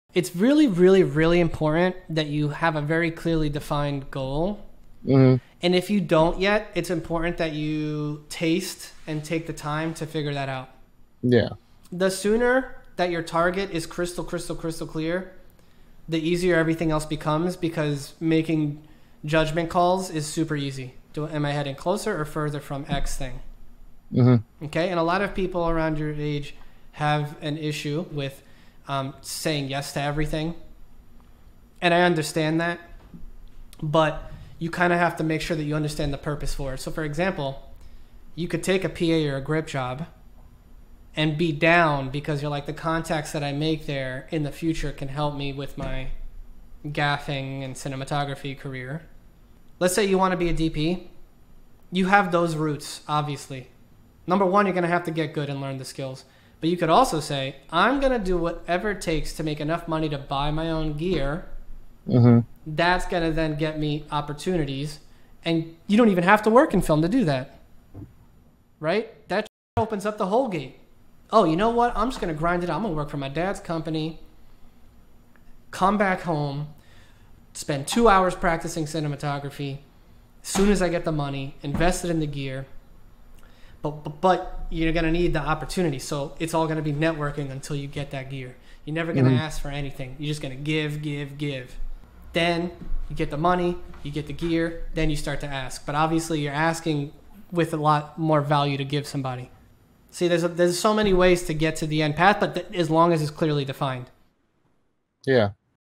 How to Define Your Filmmaking Goals
When creating goals for your future, you want as clear of a target as possible. This will make it a lot easier to evaluate all your decisions by asking "will this help get me closer to that goal?" Sometimes you'll find a job that directly leads you to that goal, but keep an open mind to all kinds of opportunities. Ex: taking a job purely for networking opportunities or to build a new skill you think will be valuable.